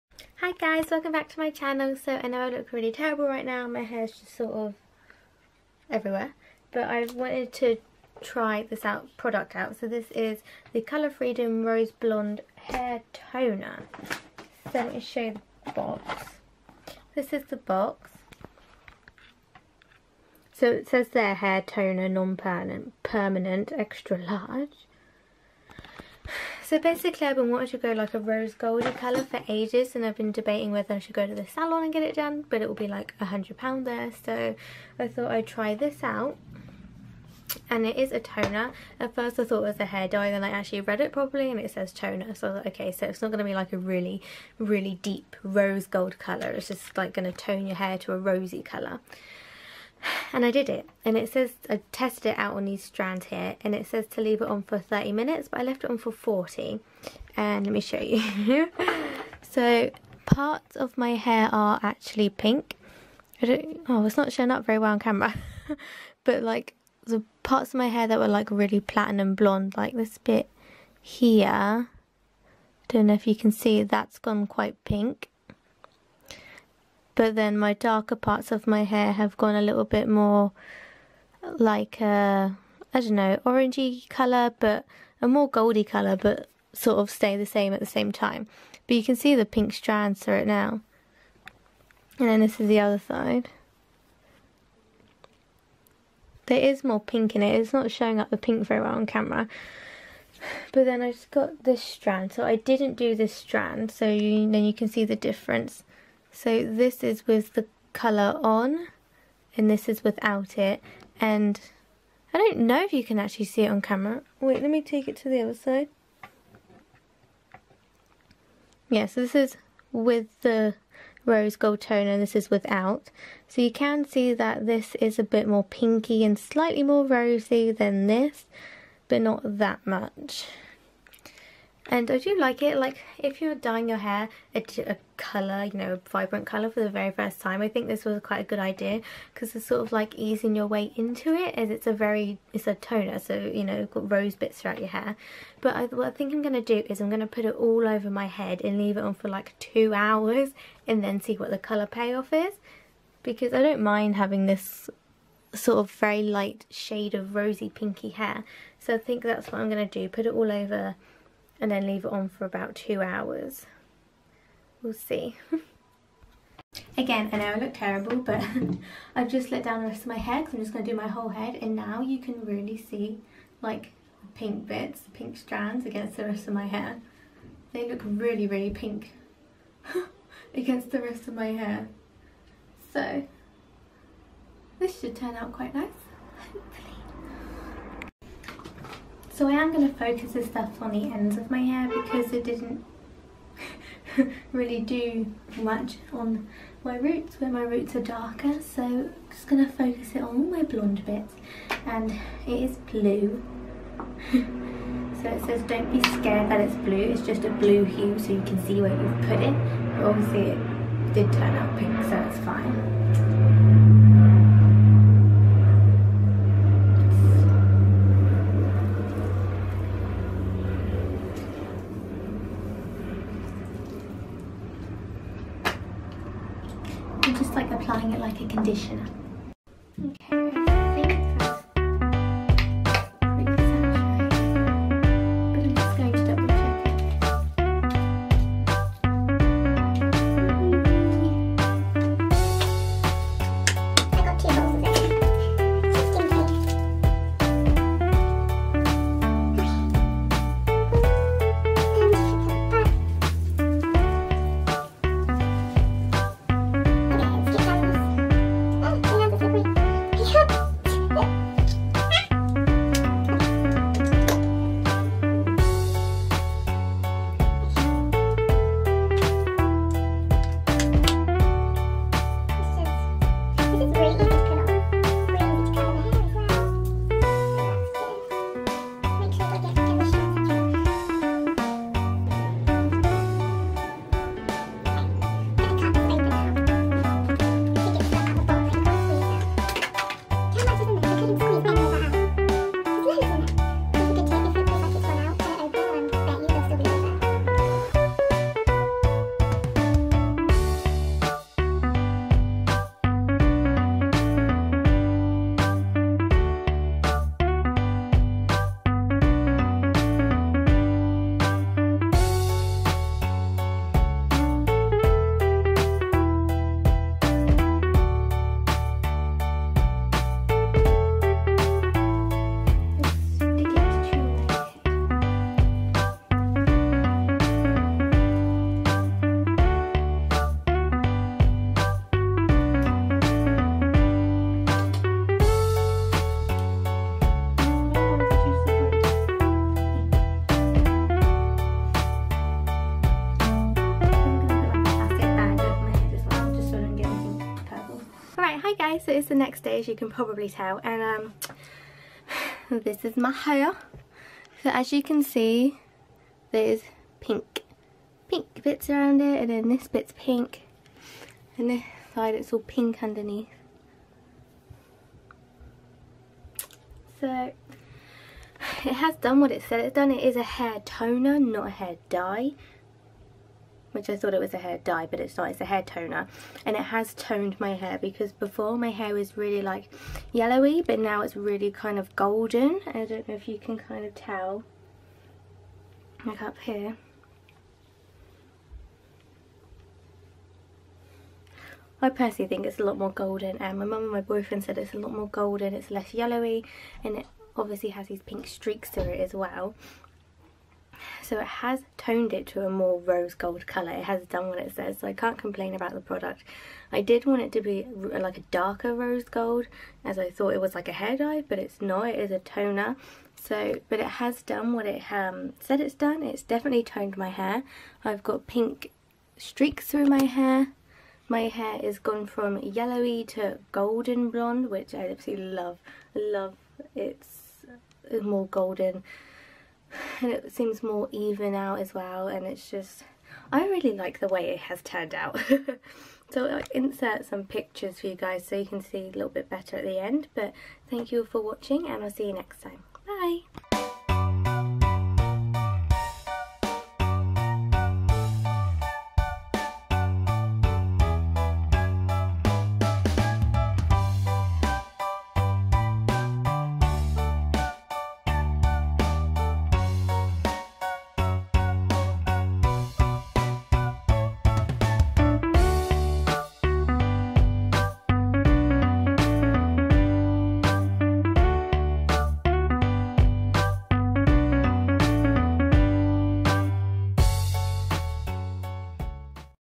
Colour freedom Rose blonde hair toner
Hey guys so today i tried out the product colour freedom rose blonde hair toner! SO when i brought this i thought it was a hair dye. as ive been wanting to go rose gold, and when i got home i read it properly and it said toner! but i love the results it defiantly has done what it says it does, my hair was a yellow blonde and after putter this on it has turned into a golden blonde and i have rose/pink sort of highlights throughout my hair and i love it, but i will still continue my journey to rose gold hair so if anyone knows of any good salons let me know down below!
Thanks
Pealbeauty01